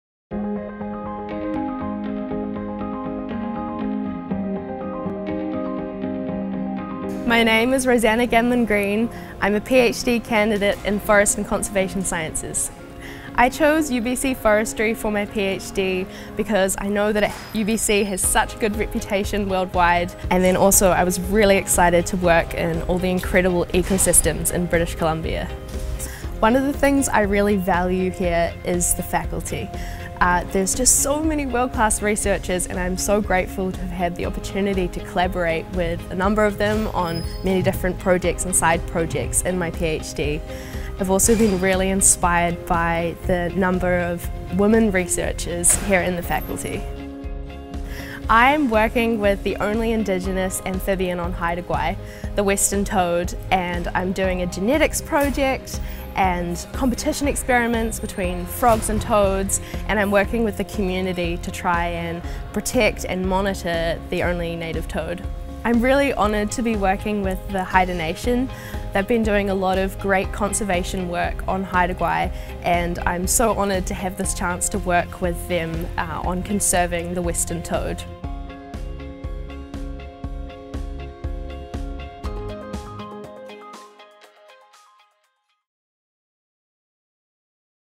PhD Doctor of Philosophy at UBC Faculty of Forestry
PhD in Forestry students can be found all over the world; they are important ambassadors for UBC Forestry and demonstrate just how diverse and international in scope the research is that is undertaken in our Faculty.

We have approximately 140 doctoral students in any given year, which is just over half of our total Forestry graduate student population.

Time Stamps
0:00 - Doctorate of Philosophy
0:13 - Why choose UBC Forestry for your PhD?
0:26 - The world-class faculty at UBC Forestry
0:48 - Roseanna's research at Haida Gwaii
1:05 - What excites you about your PhD research?